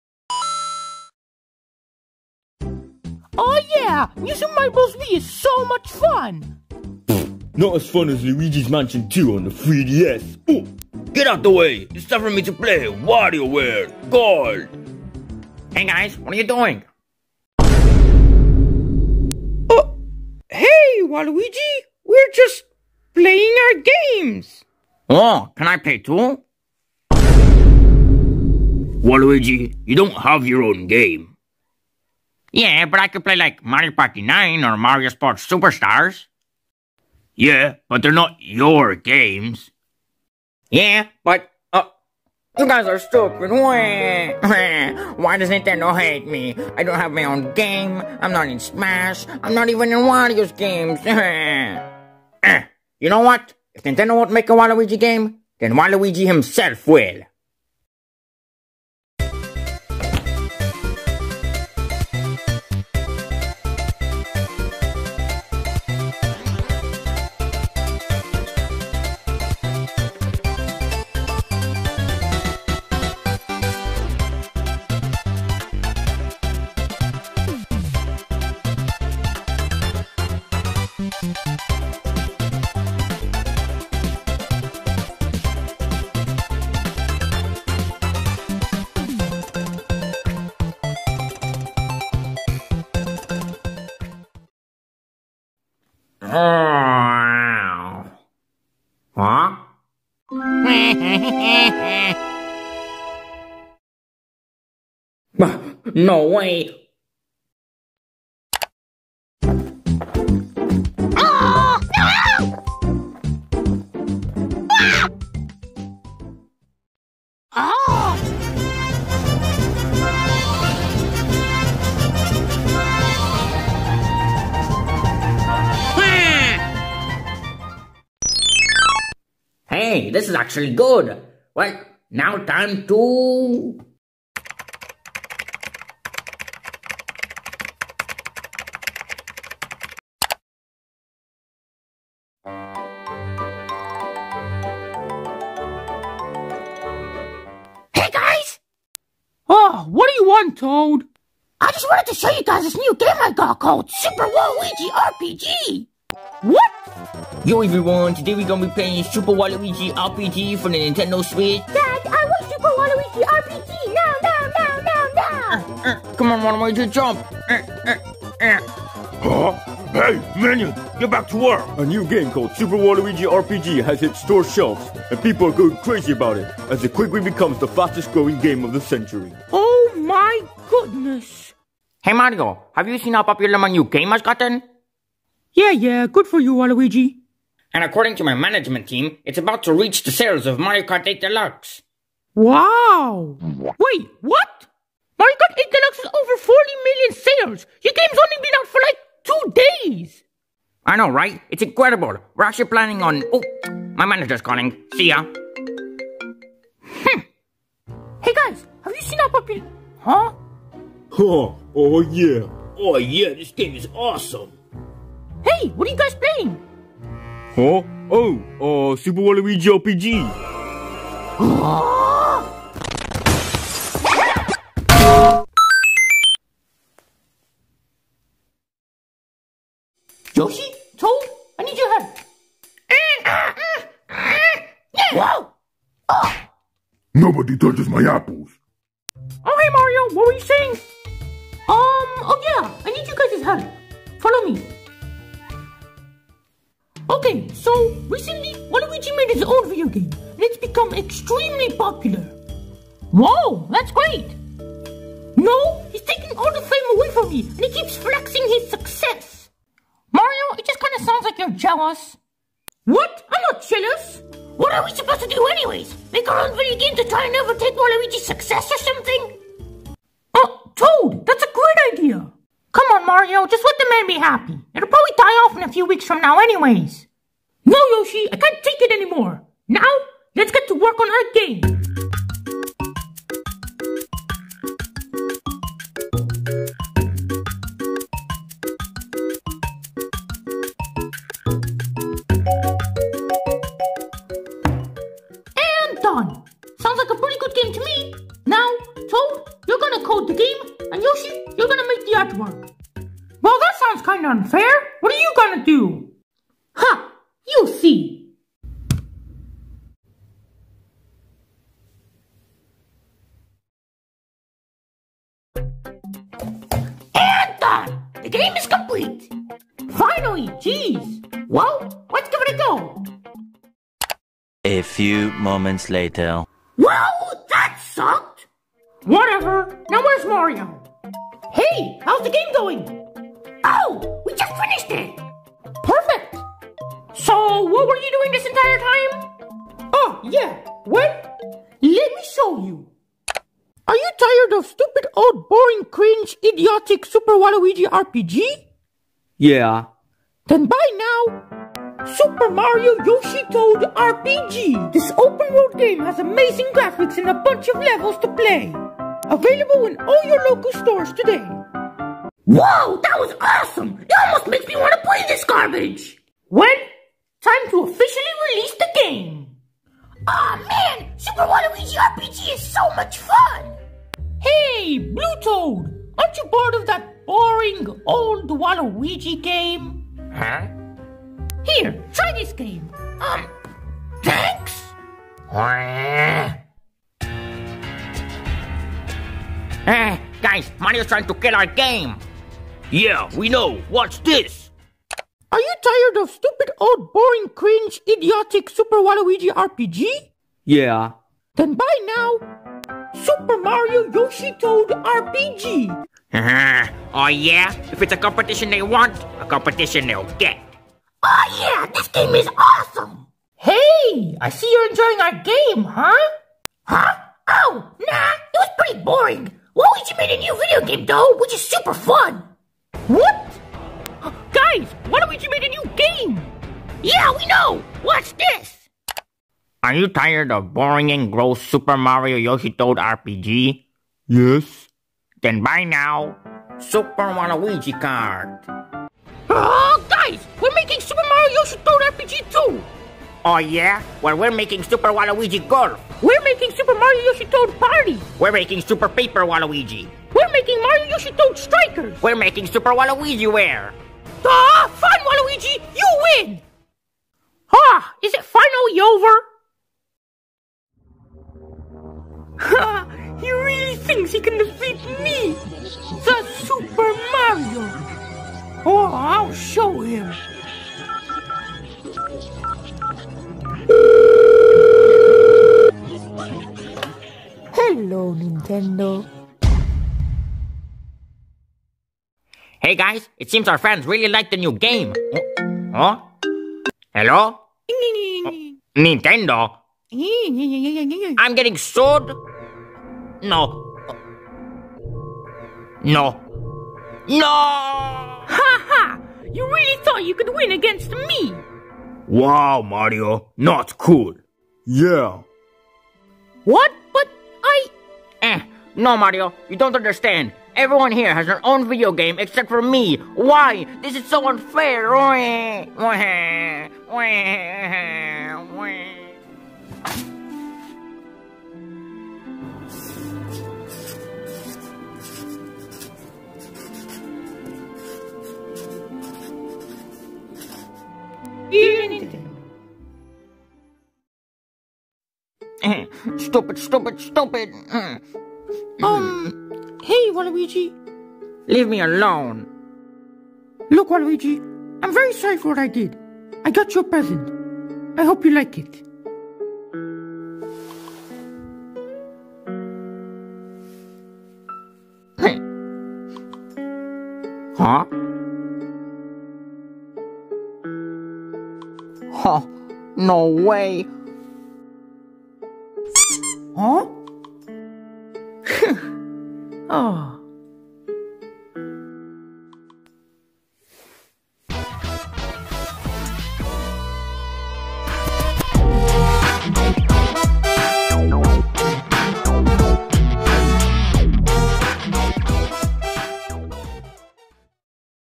Waluigi's Video Game! - Sur Mario Alex
Since Nintendo wont make a Waluigi Video Game, Waluigi decides to make his OWN game, and it goes Viral! However, Mario is jealous of his success, so he, aswell as Yoshi and Toad, make their own game, to try and OVERTAKE Waluigi, Wario and Luigi!

Video made by SurMarioAlex , Sur Mario Alex , SMA , Plush video shorts skits!